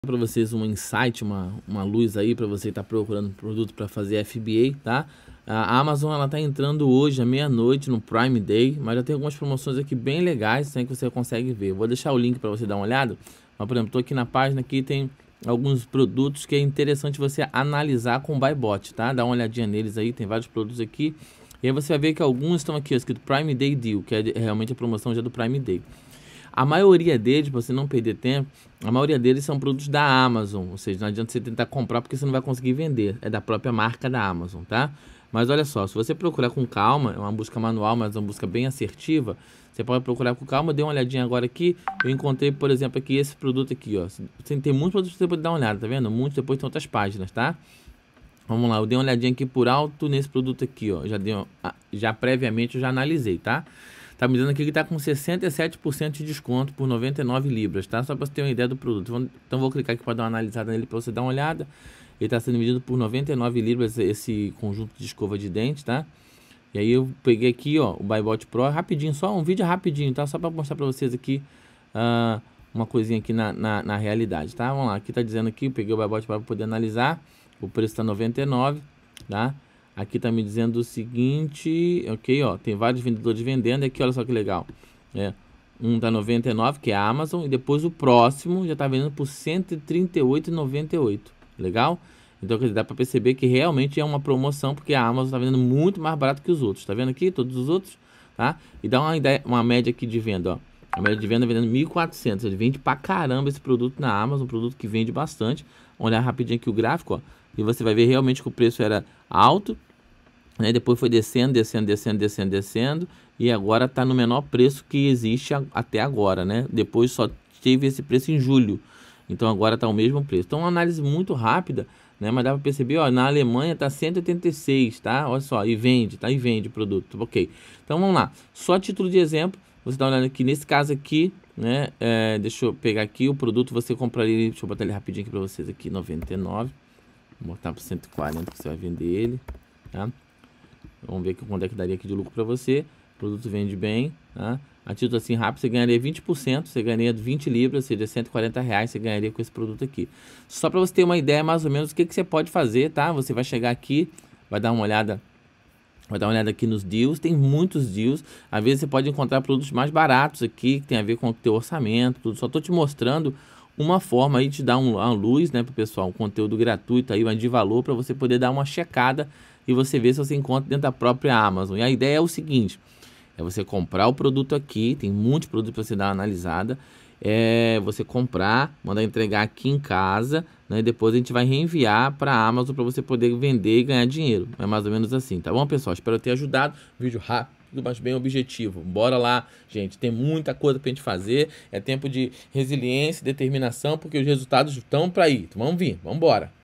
ESPECIAL #003 - Prime Day Amazon UK - Como economizar (Link na Descrição)
Prime Day Amazon UK - Como economizar, veja como pode aproveitar a promoção e sair na frente da concorrência.